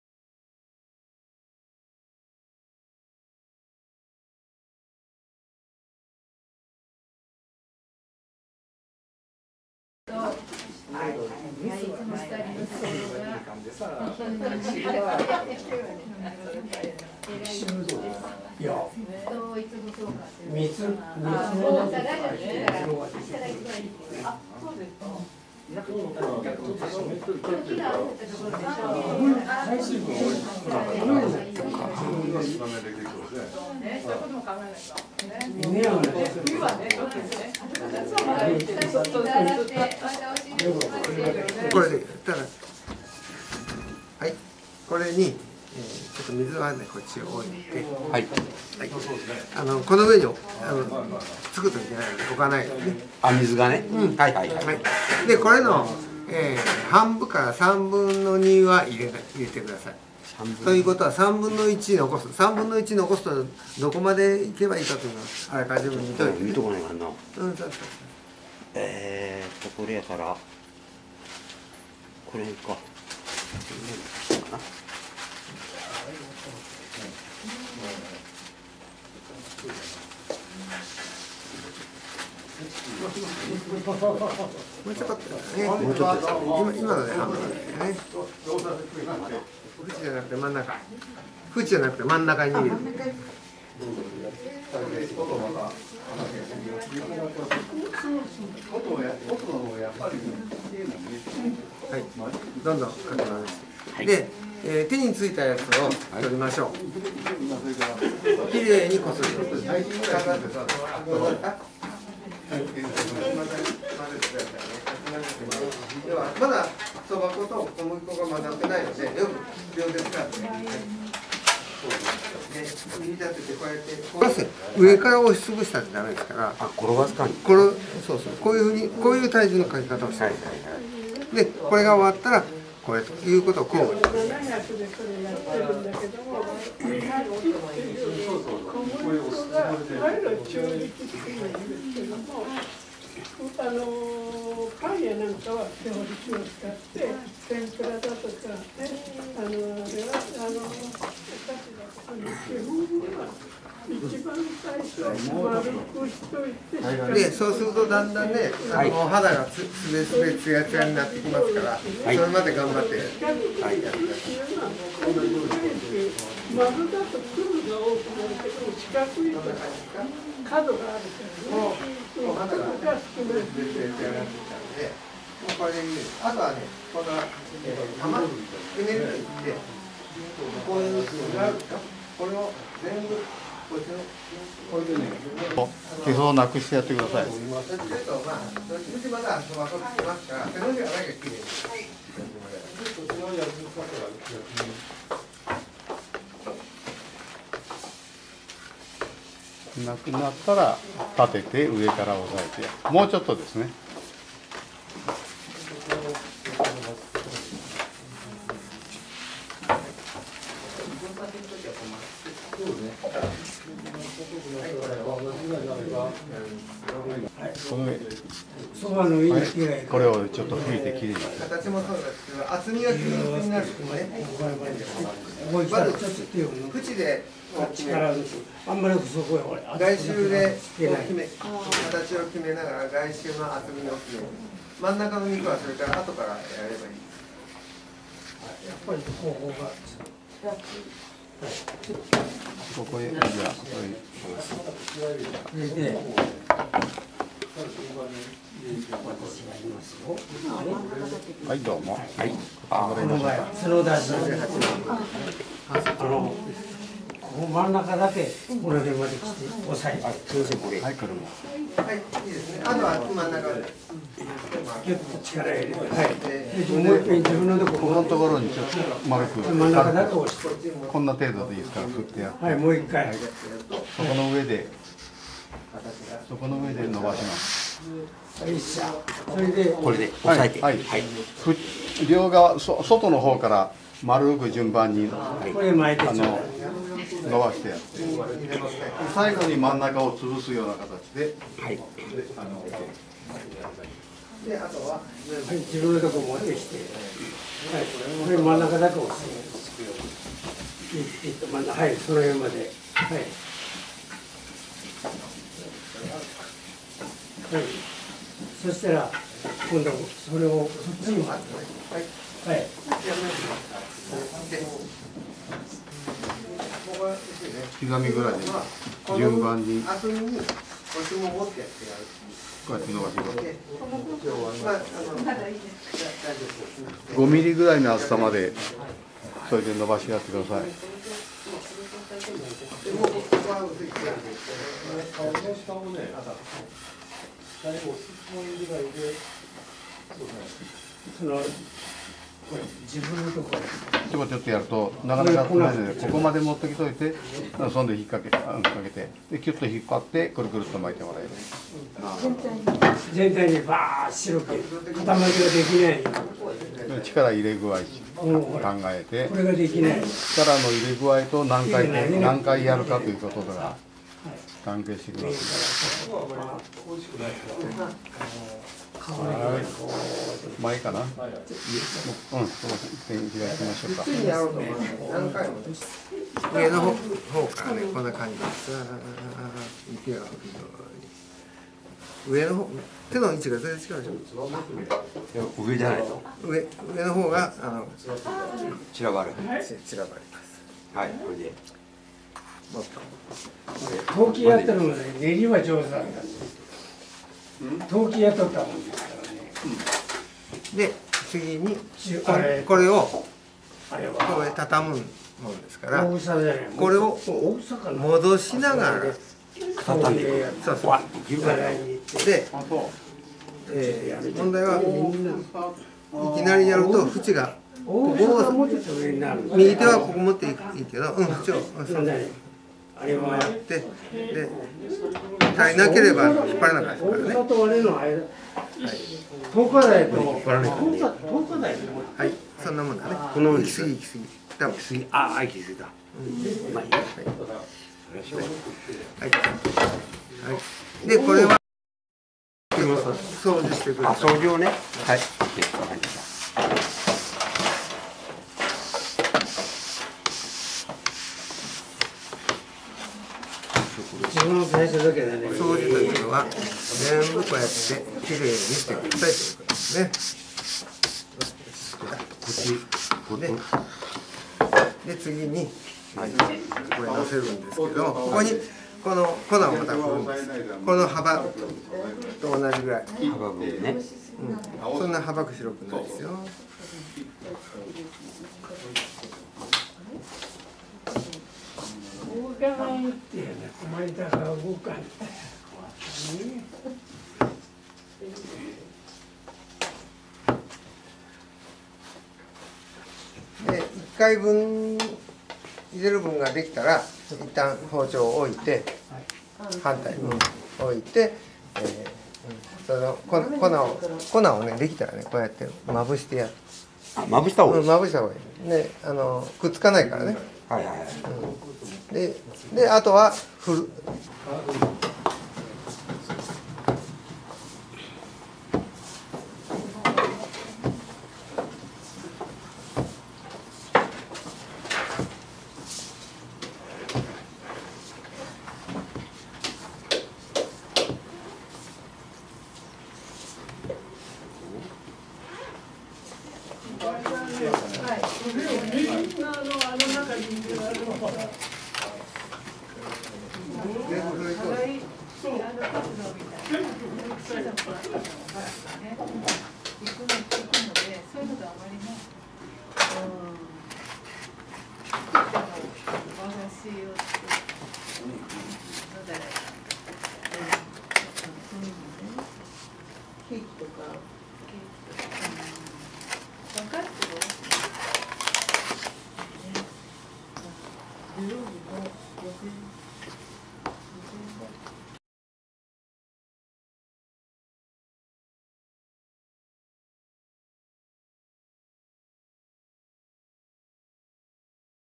Making soba noodles
Shooting is December 27
The subject is me

手打ちそば作り
撮影は12月27日です